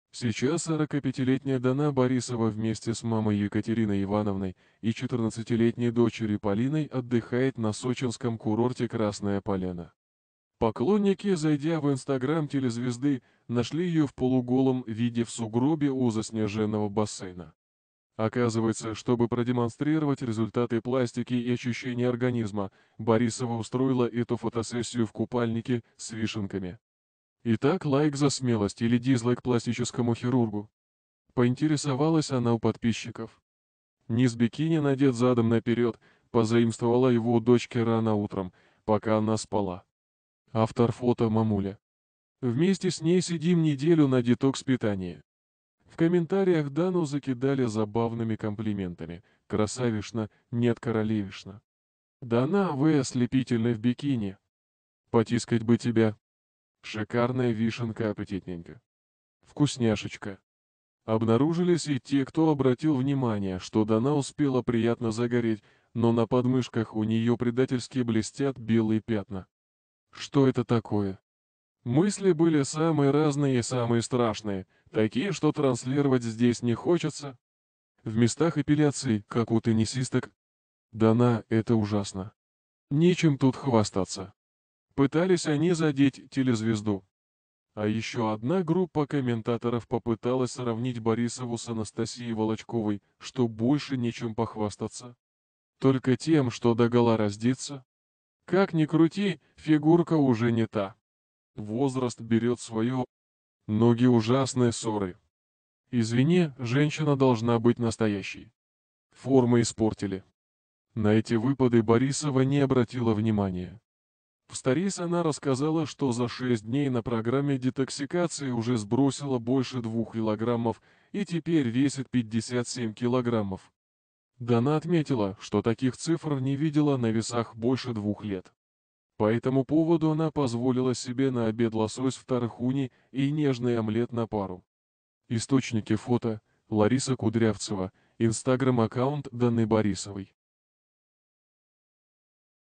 Дану Борисову нашли в сугробе в детских трусах задом наперед
Мир знаменитостей - известные люди, скандалы и события. Всё самое свежее на нашем канале.

Вся информация, использующаяся в видеосюжетах, взята из открытых источников.